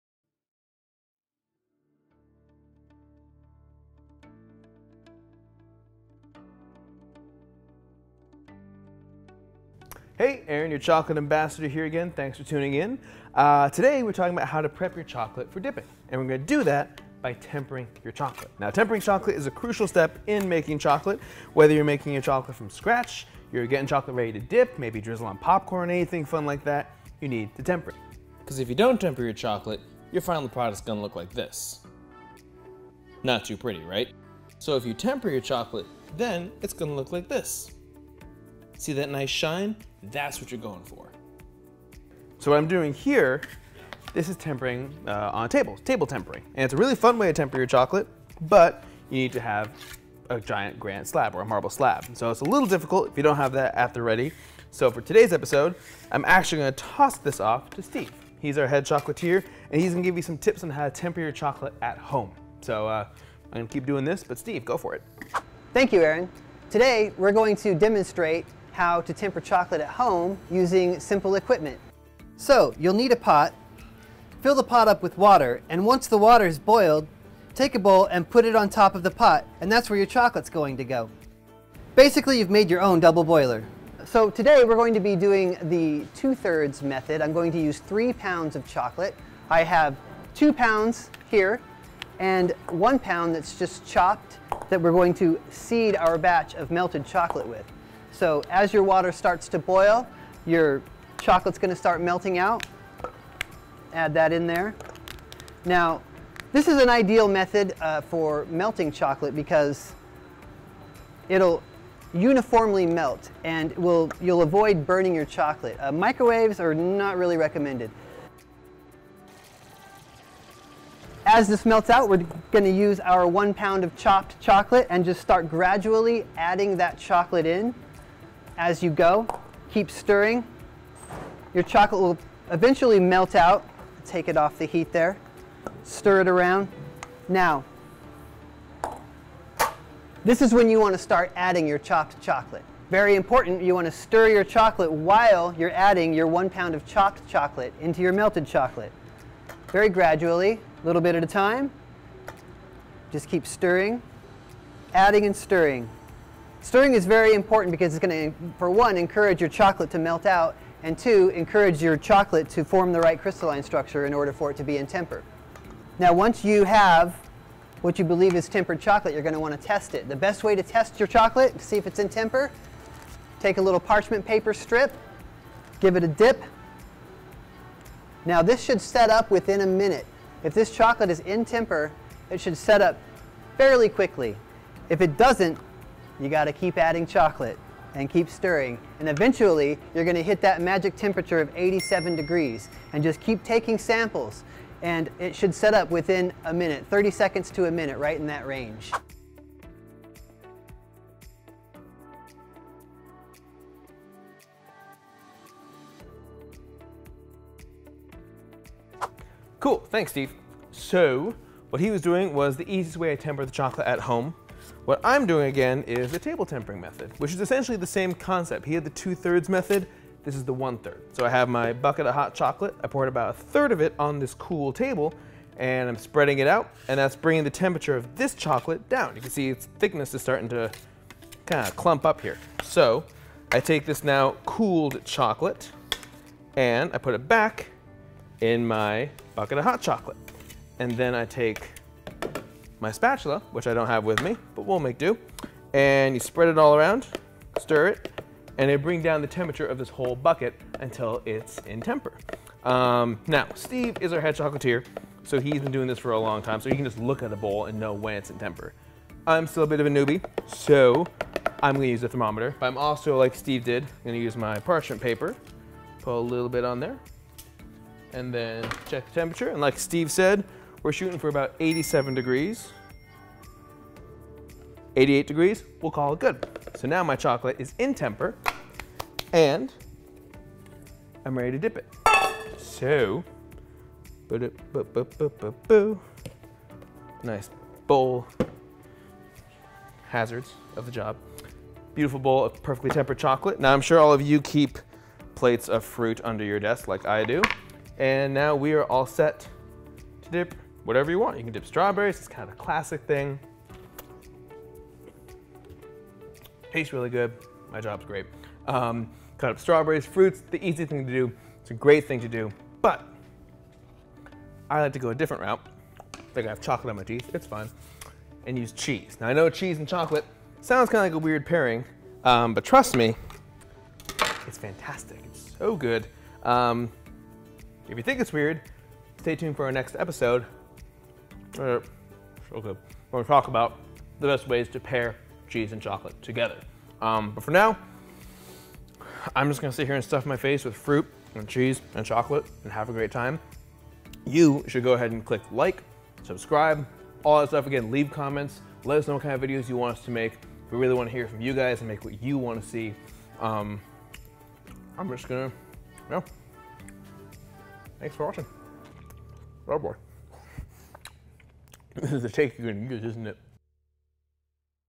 How To Prep Your Chocolate For Dipping
Looking to drizzle chocolate over popcorn? Maybe make a bowl of chocolate to drip fruit or cheese in? Well, before you can do any of that, you need to temper your chocolate! Let our chocolate ambassador and head chocolatier show you the easiest ways to do this from your home kitchen!